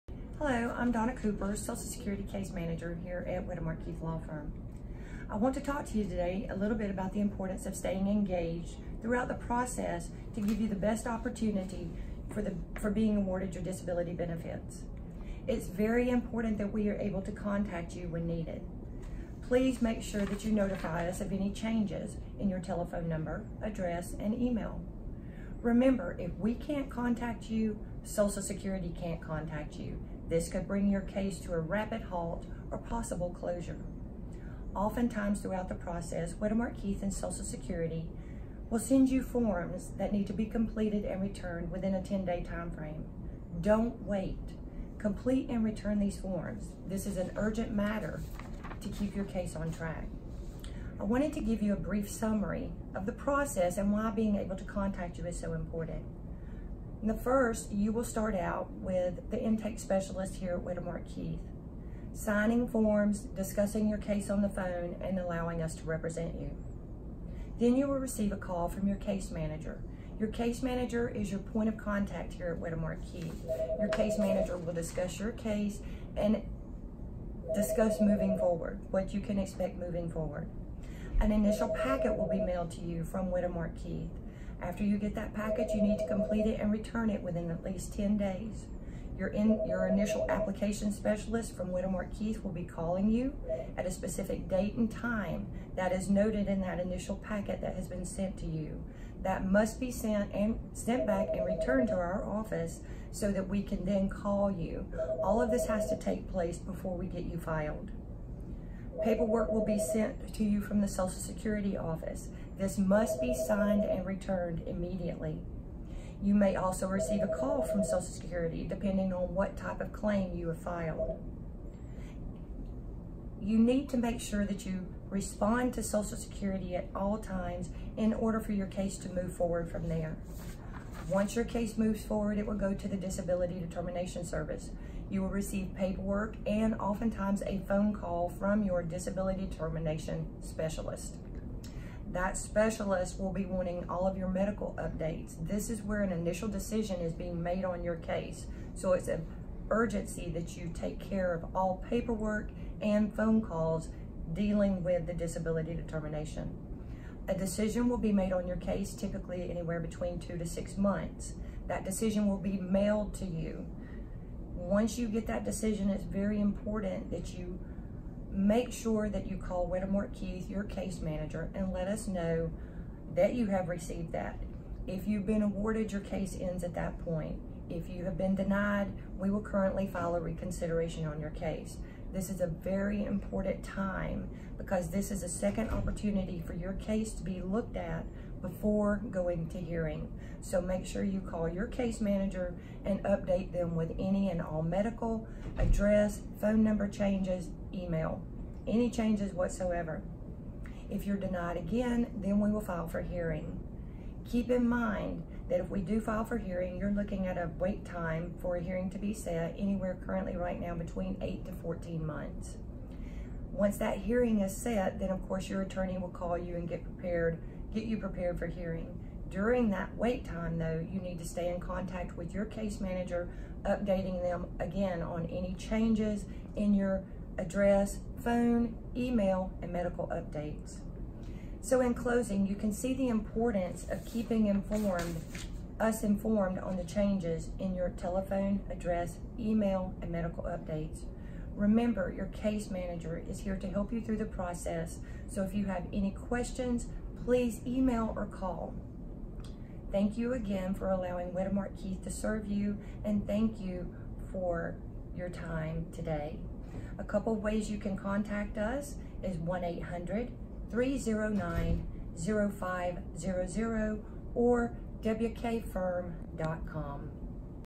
Case Manager Donna Cooper: The Importance of Staying Engaged In the SSDI/SSI Disability Process
WK Social Security case manager Donna Cooper discusses the importance of staying engaged with the firm as your case makes it's way through the SSDI/SSI decision making process.

If you have questions, please contact Wettermark Keith at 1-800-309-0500 or at wkfirm.com.